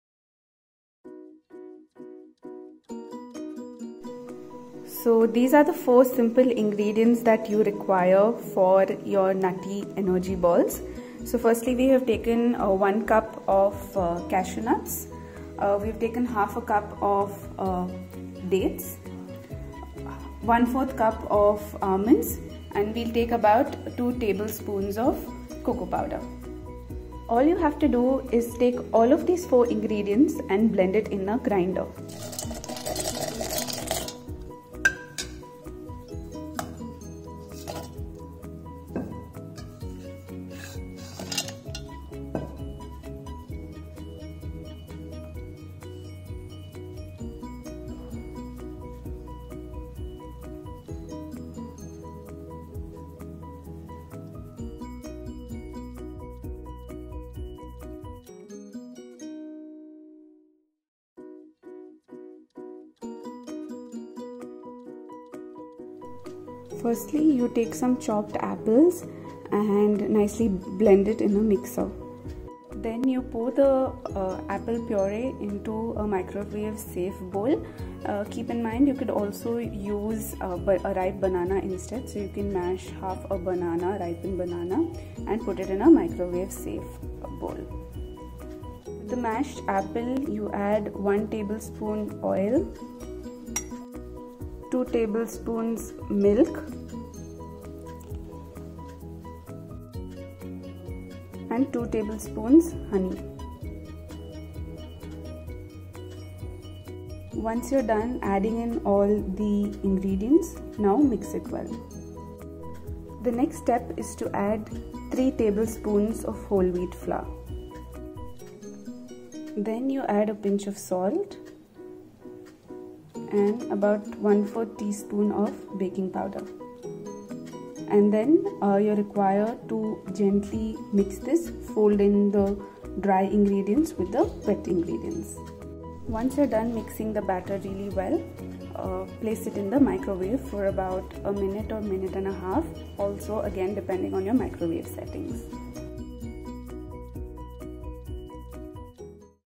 Nutty energy balls & Healthy apple mug cake recipe by Junior chef
Junior Chef: Harshita Chalam